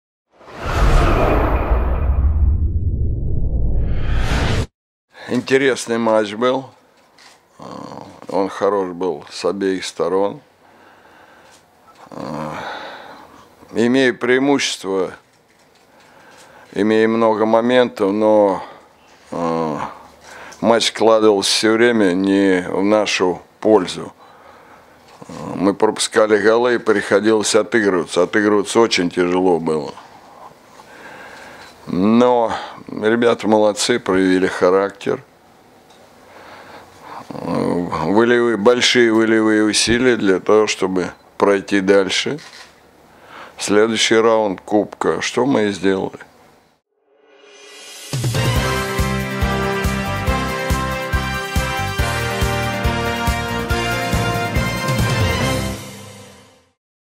Юрий Сёмин: Тяжело отыгрывались в матче с «Балтикой»
Главный тренер «Локомотива» Юрий Сёмин о матче с «Балтикой» (3:2) в 1/16 финала Олимп Кубка России.